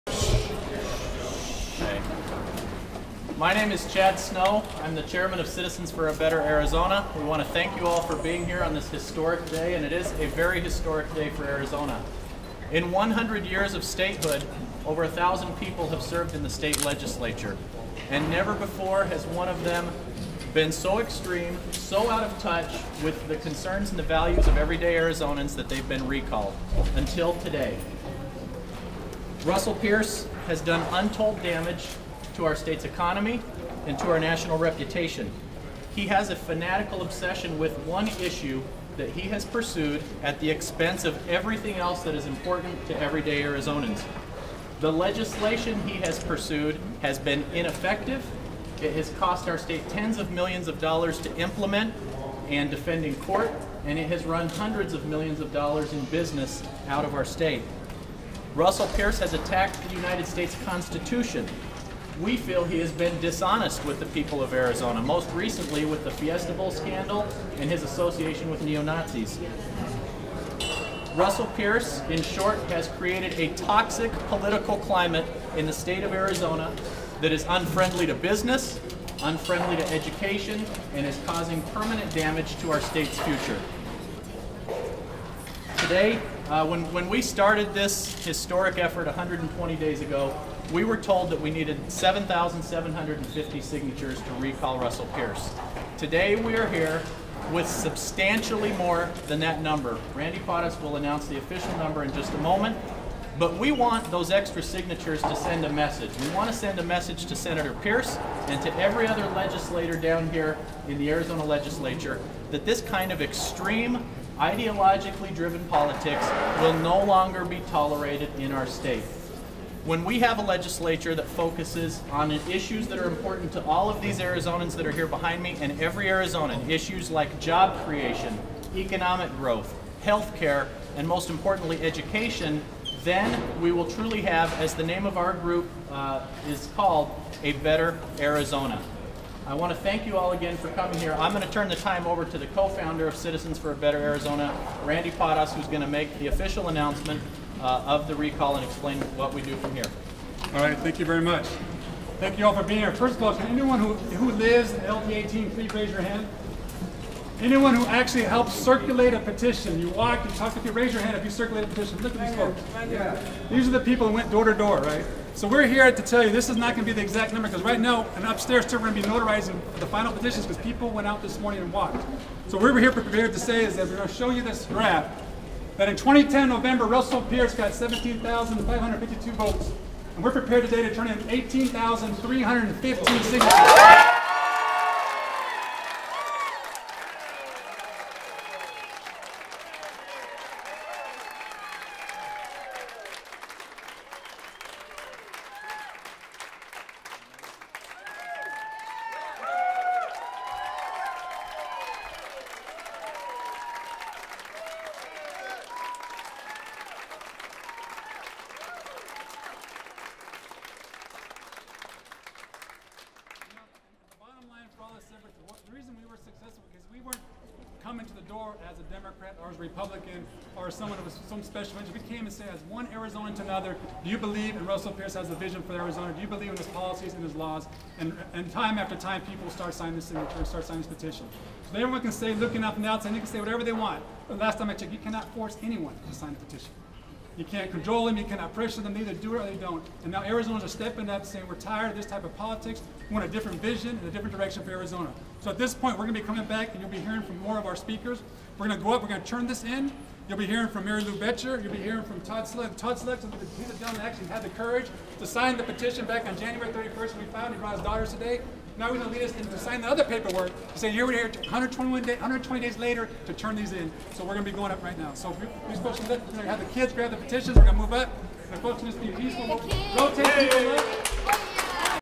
05/31/2011 - Russell Pearce Recall - Chad Snow and Randy Parraz Announce the Number of Petitions
On May 31st, 2011 Citizens for a Better Arizona submitted 18,315 petition signatures to initiate a recall of Arizona State Senate President Russell Pearce.